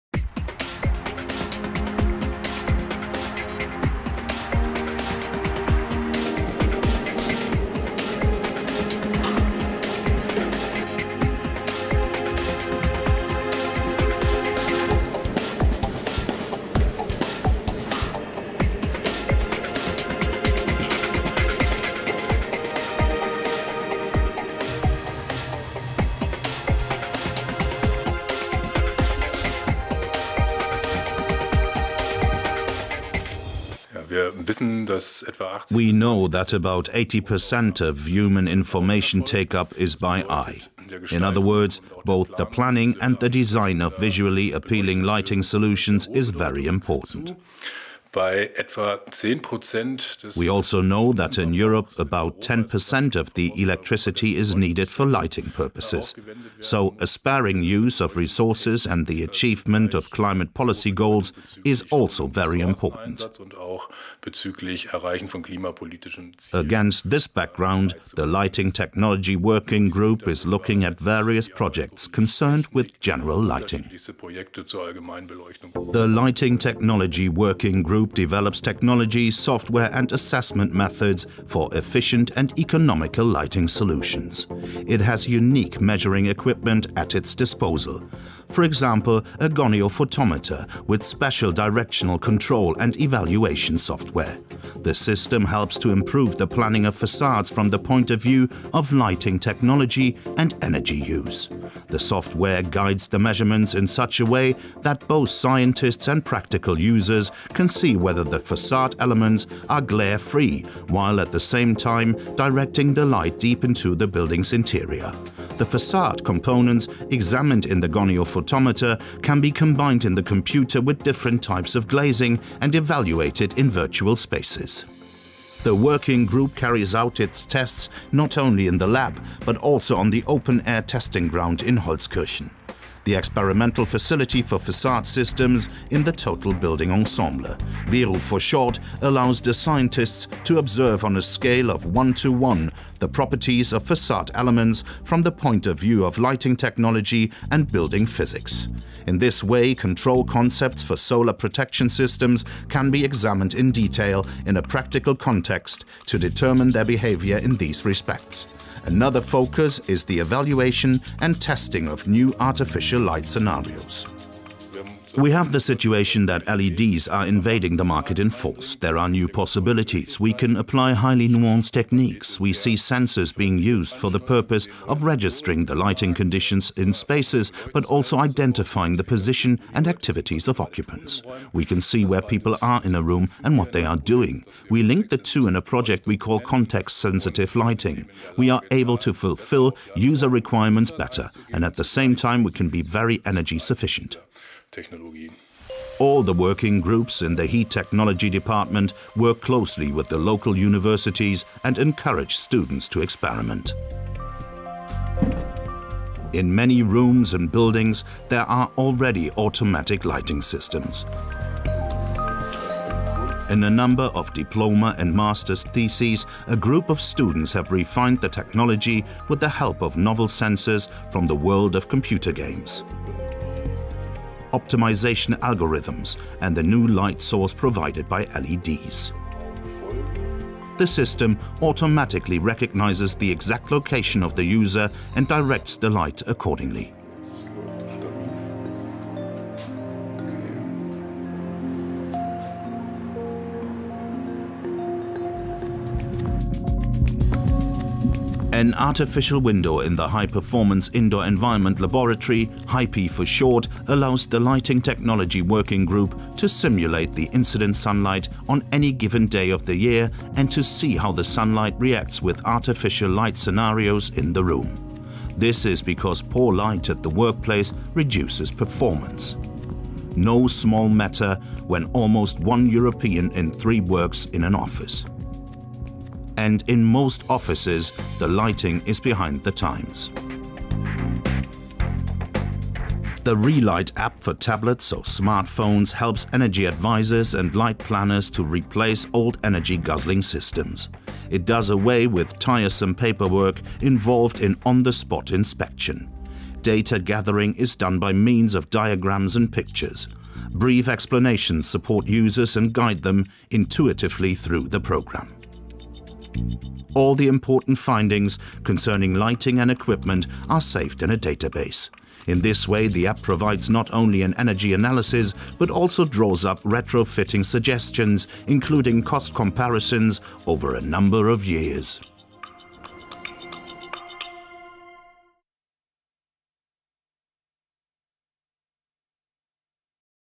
Fraunhofer IBP - Department Heat Technology - Lighting Technology Working Group
The human being perceives approximately 80-90 percent of information visually. Great significance is assigned to the configuration and planning of the visual environment that suits this requirement. At the same time, lighting engineering solutions are gaining primacy in energy efficiency. With approximately 10 % total electricity consumption in Europe used for lighting purposes, efficient lighting solutions can make a substantial contribution to conservative resource utilization and achieving the goals of climate policy. Against this background, the Lighting Technology working group develops projects for the general lighting needs of buildings and for street lighting. In this respect, the focus is placed on evaluation methods and software development, technology development, validation studies, consulting and in knowledge transfer -- as well as in the area of measurement and testing.